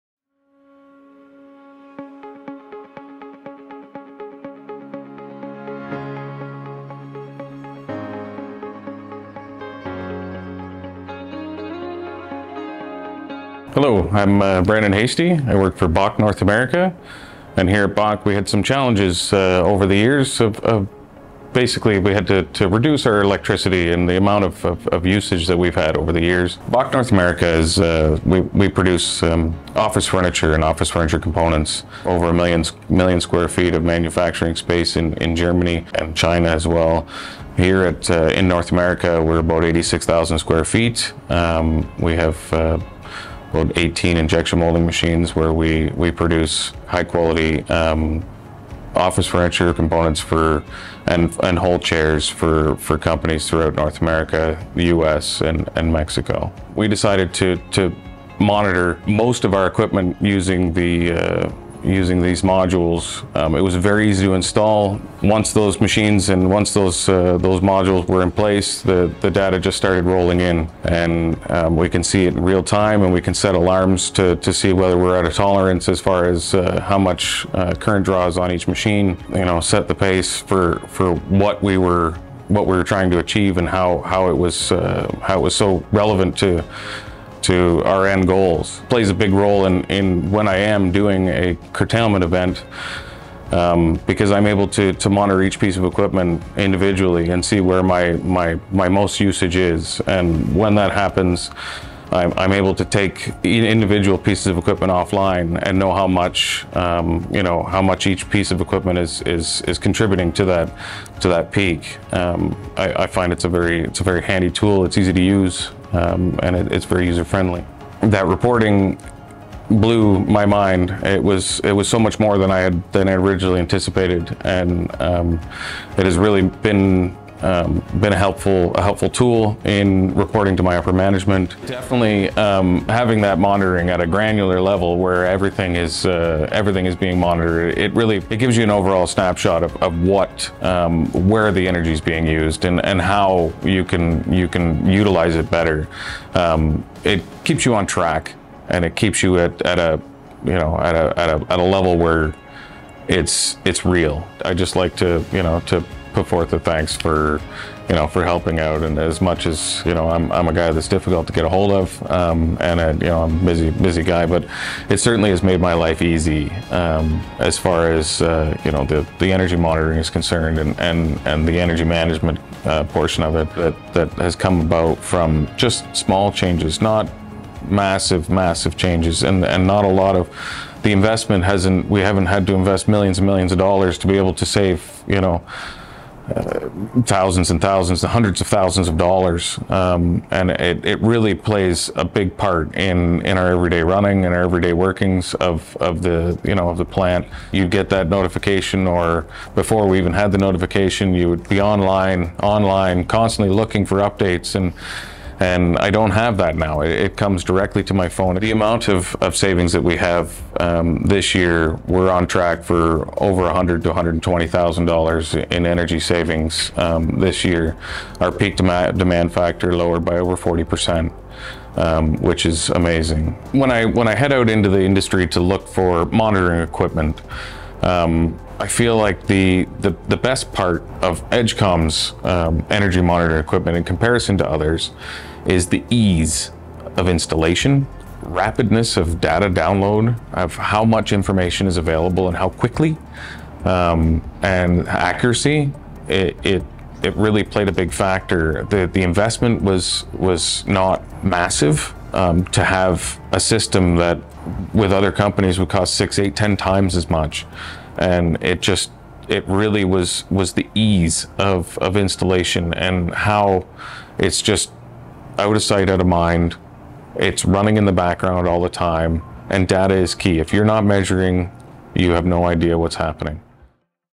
Bock North America Saves Over $100K in Energy Costs with dataTrack™ and pTrack® from Edgecom Energy
📈 Watch this inspiring success story as Bock North America, a leading company in the industrial sector, revolutionizes their energy management approach with dataTrack™ and pTrack® solutions from Edgecom Energy! 💡

In this video, you'll witness how Bock North America harnessed the power of cutting-edge technology to achieve remarkable energy cost savings of over $100,000! 🔋💰

🌟 Key Highlights:
🔹 Discover the challenges Bock North America faced in their energy management.
🔹 Explore how dataTrack™ and pTrack® from Edgecom Energy revolutionized their energy monitoring and control.
🔹 Learn about the real-world results, including the significant savings achieved.
🔹 Hear from Bock North America about their experience and the impact on their operations.

If you're looking for innovative ways to optimize your energy consumption, reduce costs, and enhance your sustainability efforts, this video is a must-watch! 🌎

Don't miss out on this incredible success story – hit that 'Like' button, 'Subscribe' for more inspiring content, and 'Share' with your network to spread the word about the incredible benefits of dataTrack™ and pTrack® from Edgecom Energy!